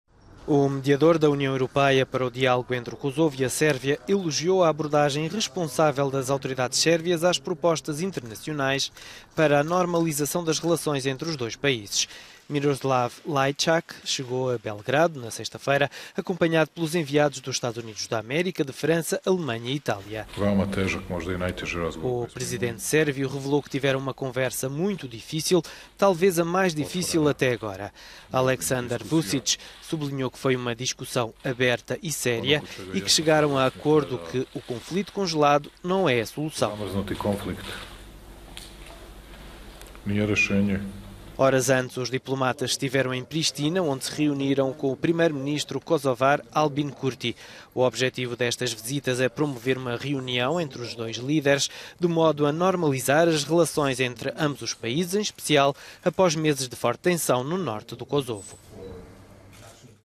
UE elogia abordagem "responsável" da Sérvia
Presidente sérvio diz que conflito "congelado" com o Kosovo "não é a solução". Mediador da União Europeia elogia abordagem "responsável" de Belgrado